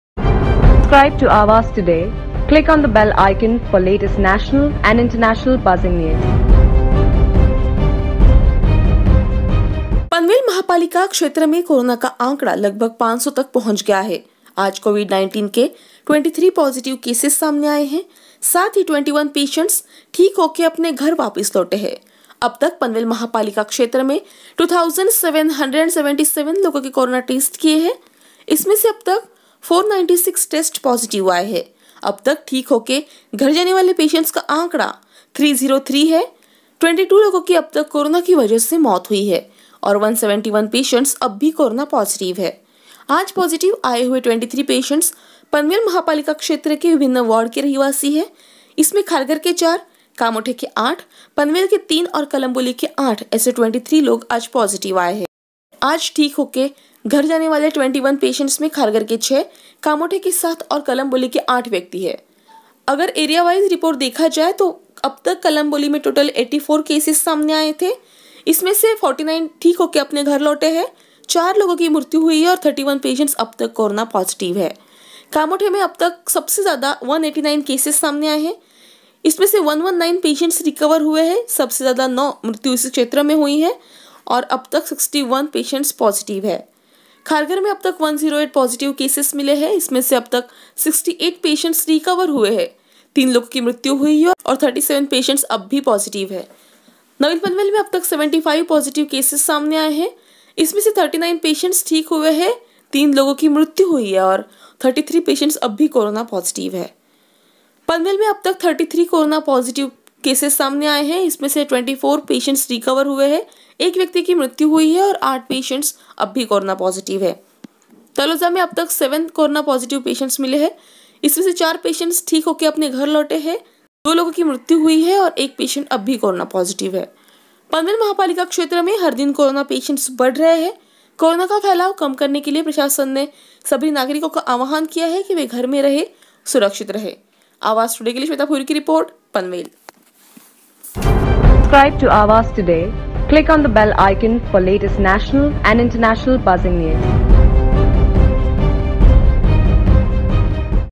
Panvel has 500 cases with 23 new Covid 19 cases and 21 recoveries - PMC May 30
पनवेल महापालिका क्षेत्र में कोरोना का आंकड़ा लगभग 500 तक पोहोंच गया है.. आज covid 19 के 23 पोझिटिव केसेस सामने आए है..साथ ही 21 पेशंट्स ठीक होके अपने घर वापिस लौटे है..

अब तक पनवेल महा पालिका क्षेत्र में 2,777 लोगो के कोरोना टेस्ट किये है.. इसमे से अब तक 496 टेस्ट पोझिटिव आये है.. अब तक ठीक होके घर जाने वाले पेशंट्स का आंकड़ा 303 है.. 22 लोगो की अब तक कोरोना की वजह से मौत हुई है...औऱ 171 पेशंट्स अब तक कोरोना पोझिटिव है..आज पोझिटिव आये हुए 23 पेशंट्स पनवेल महा पालिका क्षेत्र के विभिन्न वार्ड के रहिवासि है..

इसमे खारघर के 4, कामोठे के 8, पनवेल के 3, और कलंबोली के 8, ऐसे 23 लोग आज पोझिटिव आये है..

आज ठीक होके घर जाने वाले 21 पेशंट्स में खारघर के 6, कामोठे के 7, और कलंबोली के 8 व्यक्ति है..

अगर एरिया वाइज रिपोर्ट देखा जाए..
तो अब तक कलंबोली मे टोटल 84 केसस सामने आए थे इसमे से 49 ठीक होके घर लौटे है..4 लोगो की मृत्यु हुई है और 31 पेशंट्स अब तक पोझिटिव है...

कामोठे में अब तक सबसे ज्यादा 189 केसेस सामने आए है.. इसमे से 119 पेशंट्स रिकवर हुए है..सबसे ज्यादा 9 मृत्यु इसी क्षेत्र में हुई है.. और अब तक 61 पेशंट्स अभी भी पोझिटिव है..

खारघर में अब तक 108 पोझिटिव केसेस मिले है..इसमे से अब तक 68 पेशंट्स रिकवर हुए है, 3 लोगों की मृत्यु हुई है औए 37 पेशंट्स अब भी पोझिटिव है..

नवीन पनवेल में अब तक 75 पोझिटिव केसेस सामने आए है..इसमे से 39 पेशंट्स ठीक हुए है हर 3 लोगो की मृत्यु हुई है..औए 33 पेशंट्स अब भी कोरोना पोझिटिव है..
पनवेल में अब तक 33 कोरोना पोझिटिव केसस सामने आए है.. इसमे से 24 पेशंट्स रिकवर हुए है 1 व्यक्ति की मृत्यु भी हुई है और 8 पेशंट्स अब तक कोरोना पोझिटिव है..

तलोजा में अब तक 7 कोरोना पेशंट्स मिले है.. इसमे से 4 पेशंट्स ठीक होके घर लौटे है तो 2 लोगो की मृत्यु हुई है..और 1 पेशंट अब भी कोरोना पोझिटिव है...

पनवेल महापालिका क्षेत्र में हर दिन कोरोना पेशंट्स बढ़ रहे है..
कोरोना का फैलाव कम करने के लिए प्रशासन से सभी नागरिकों को आवाहन किया है के वे घर मे रहे, सुरक्षित रहे...

आवाज टुडे के लिए श्वेता भोईर की रिपोर्ट, पनवेल.